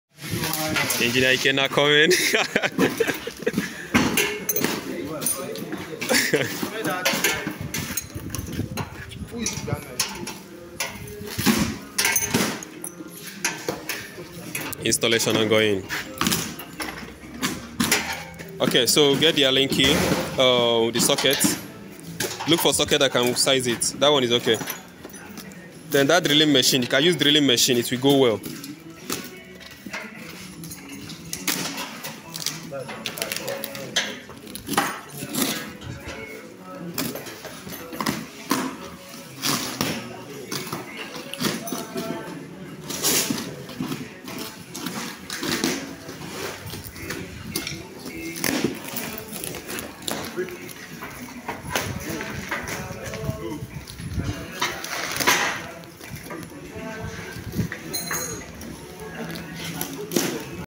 BLUESUN LITHIUM BATTERY RACK SETUP AND INSTALLATION
Project Title: Installation of 100kw Inverter, 192 Solar Panels and 20 Lithium batteries by Vynet invent limited.